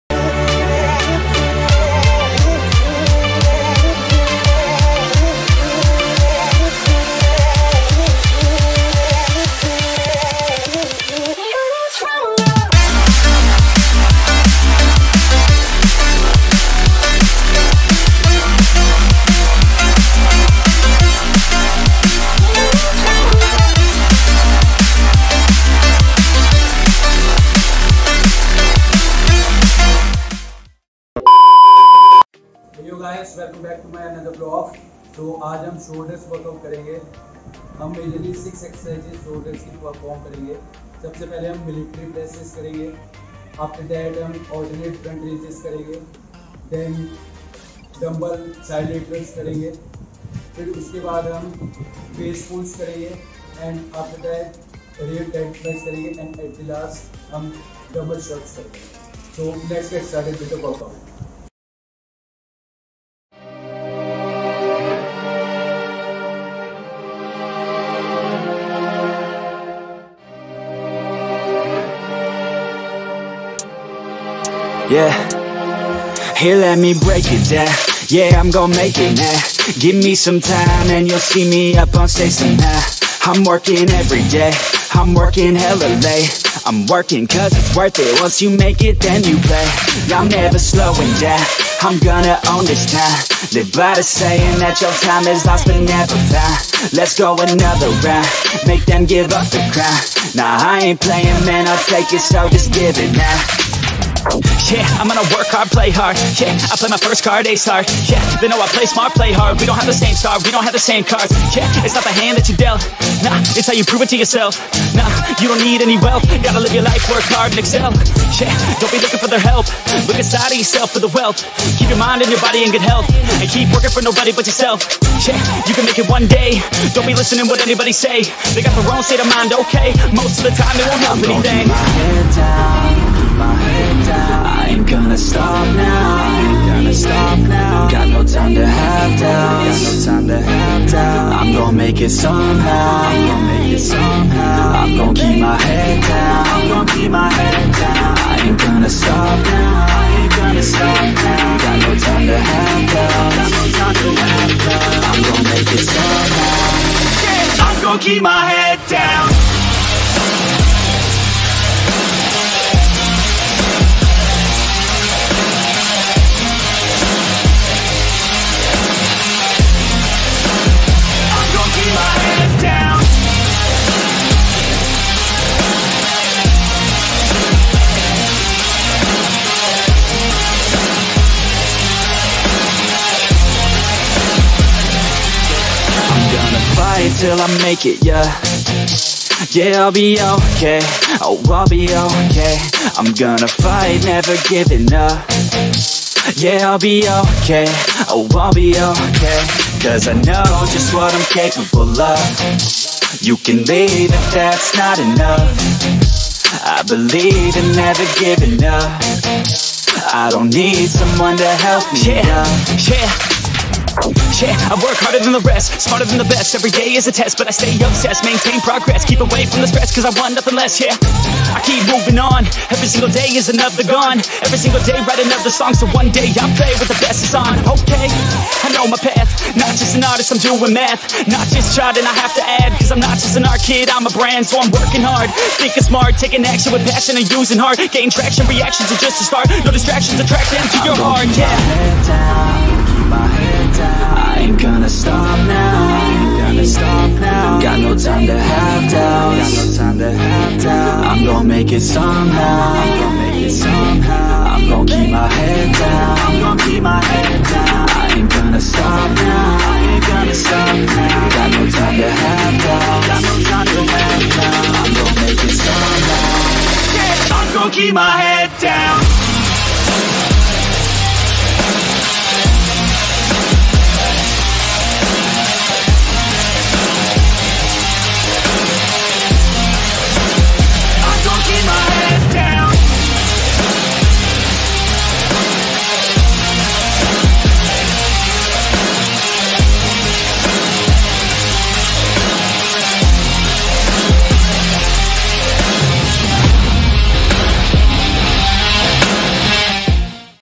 6 Best Exercises For The Overall Shoulder Growth | Boulder Shoulders Workout | Covers Three Delts
In this video I'm performing 6 shoulder exercises which covers all the three Delts.
1. Military Presses
2. Alternate Front Raises
3. Dumbbell Side Laterals
4. Face Pulls
5. Rear Delt Flyes
6. Dumbbell Shrugs

🔔 Click on the bell icon and never miss a video !